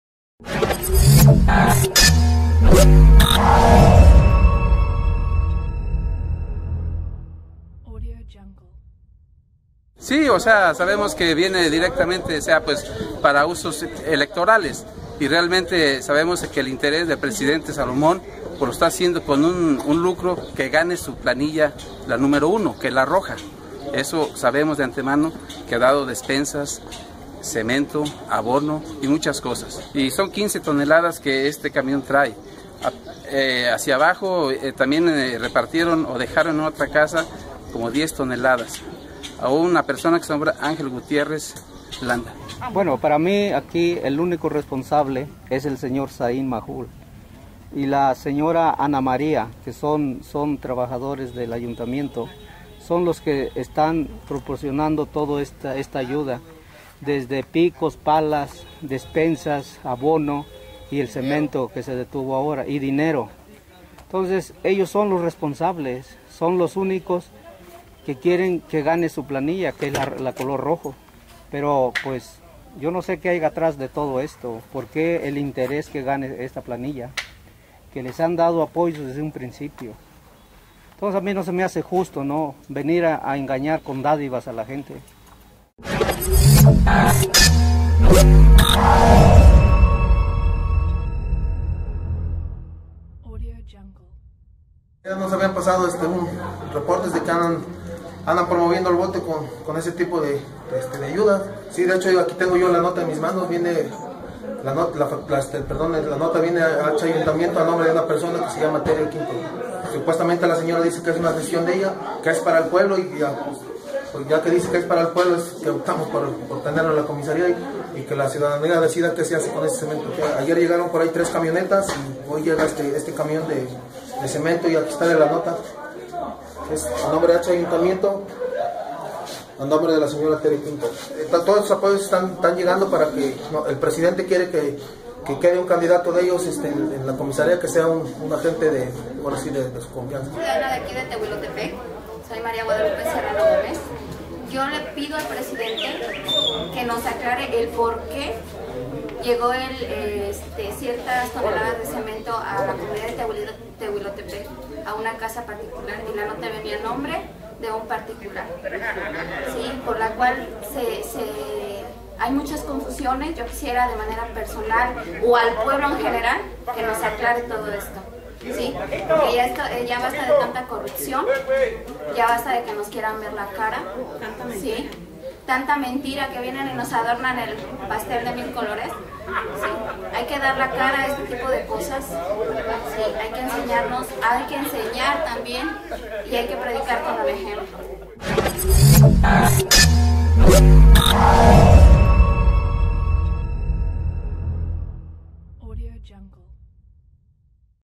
Detienen camiones de Burgos con cemento de prog. sociales para comprar votos para comisarios
Detienen en Taxco el viejo, Tehuilotepec y otras comunidades camiones de Construrama "materiales para construcción Burgos" con toneladas de cemento de programas sociales, despensas y dádivas que serían utilizadas para comprar votos en favor de los candidatos a comisarios de Salomón Majul González y Said Majul Ballesteros, así lo denuncian habitantes de esas comunidades del municipio de Taxco de Alarcon.